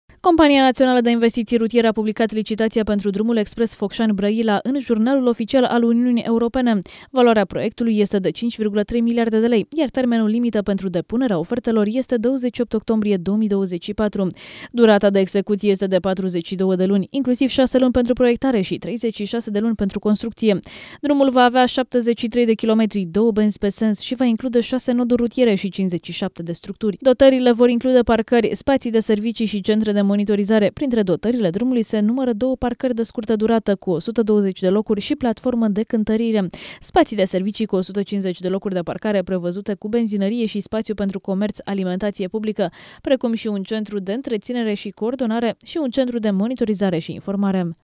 Prima licitatie lansata de CNIR - Drumul Expres Focșani-Brăila
Compania Națională de Investiții Rutiere a publicat în Jurnalul Oficial al Uniunii Europene (JOUE), licitația pentru proiectarea și execuția Drumului Expres Focșani-Brăila.